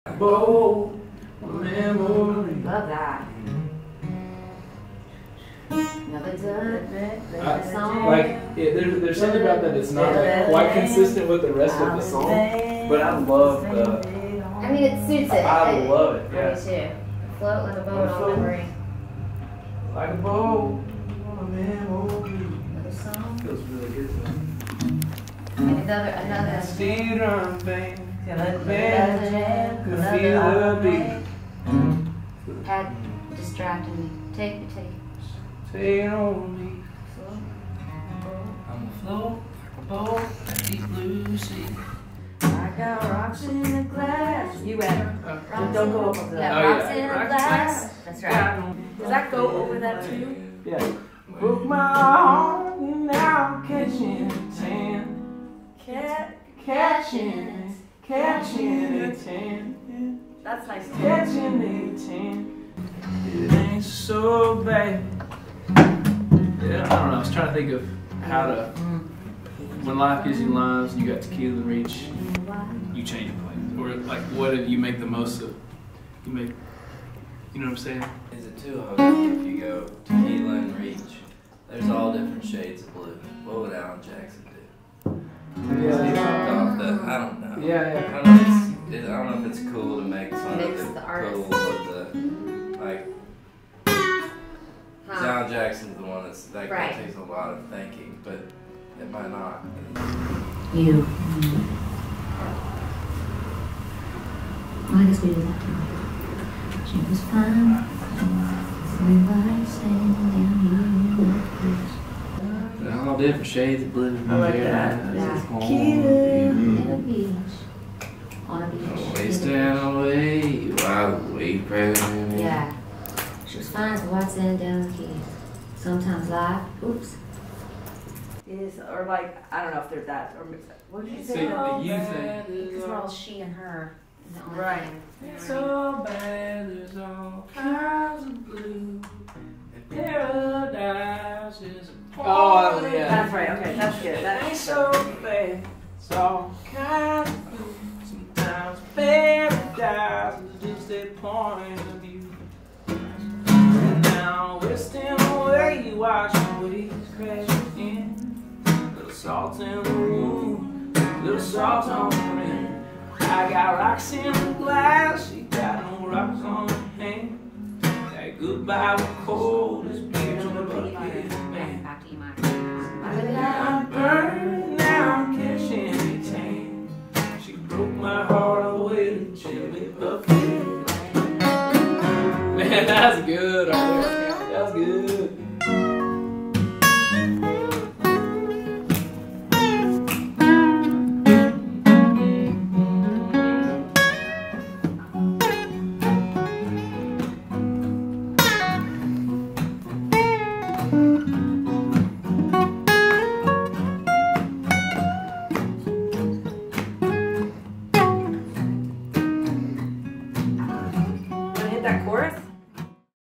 Toad Suck Daze Writers Retreat | Conway Area Chamber of Commerce
We got a behind-the-scenes look at last week's Toad Suck Daze Writers Retreat. We think you'll enjoy it. The retreat was a two-day intimate gathering with a handful of Nashville songwriters  — including Kelly Archer, Dylan Guthro and Jenn Schott — at the University of Central Arkansas with Toad Suck Daze creative director, and fellow songwriter, Adam Hambrick

Thanks to Banded, The Bluebird Cafe and the University of Central Arkansas for making this possible! We already cannot wait for the next one!

ToadSuckDaze | Chris Crain Hyundai | Crain Genesis of Conway | Gainwell Technologies